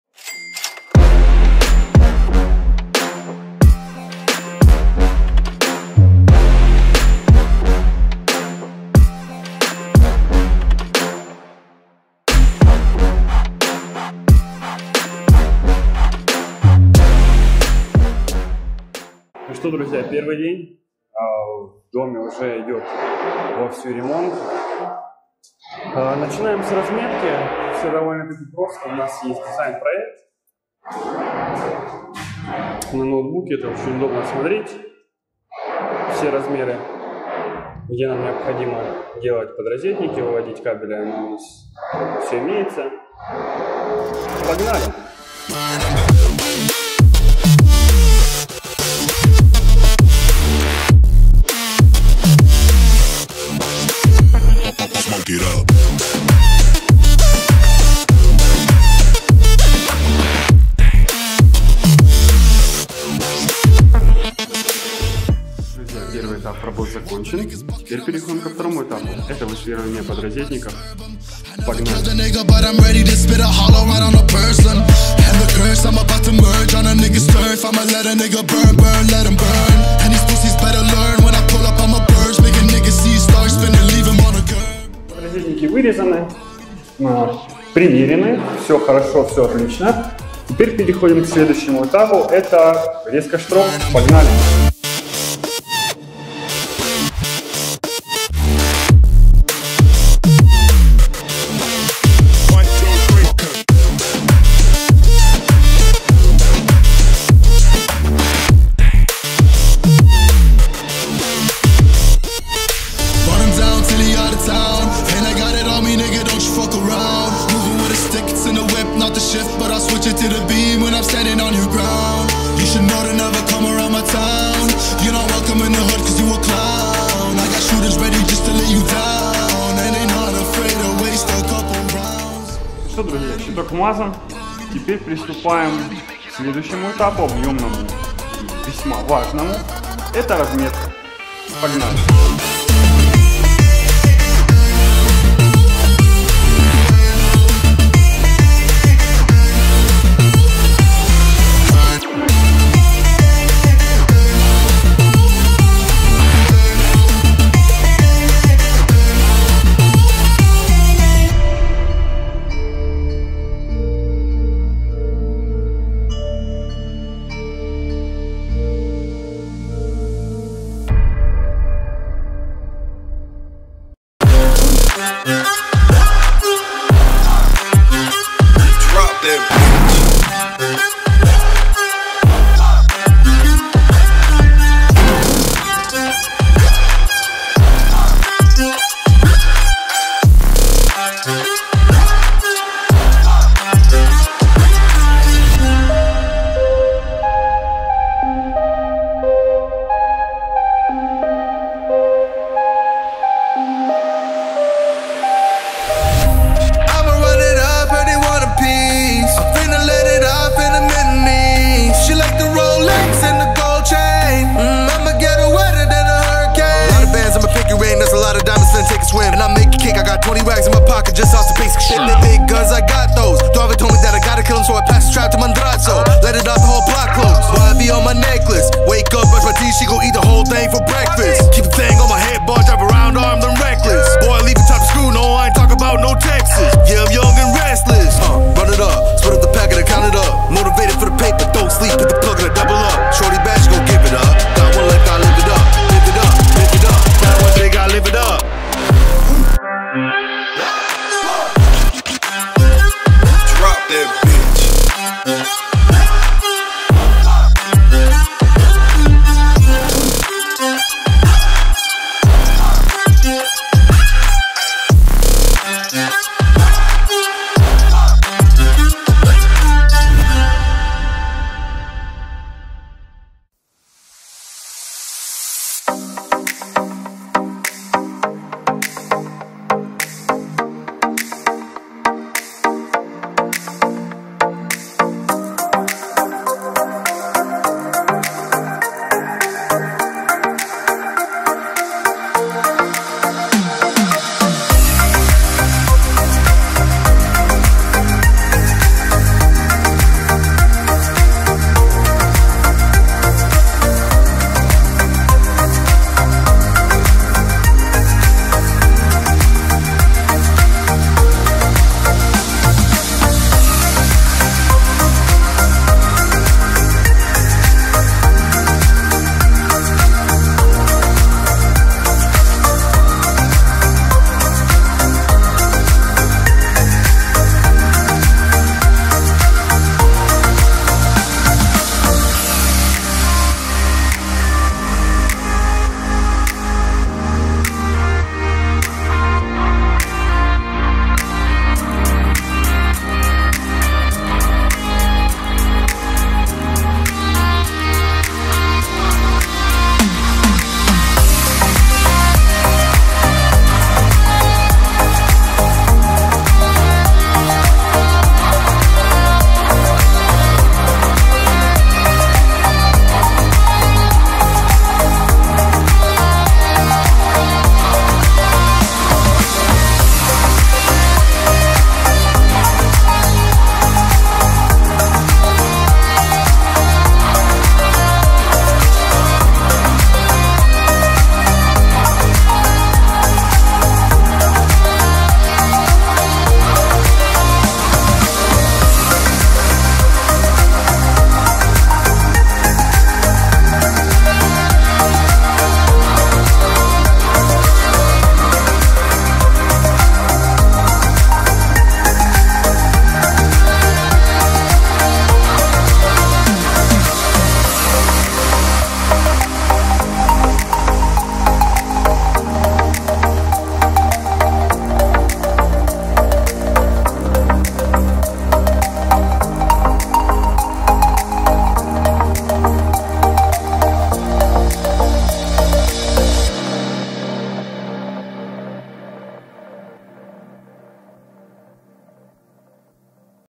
Электрика в трёхкомнатной квартире от первого лица! Щит ABB UK600, автоматика DEKraft.
Приветствую Вас друзья!
В данном видео я покажу, как я делаю электрику под ключ в Ваших домах и квартирах. Вся съёмка с экшн камеры от первого лица)

Заказать электромонтажные работы в Ростове-на-Дону можно по номеру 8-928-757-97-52.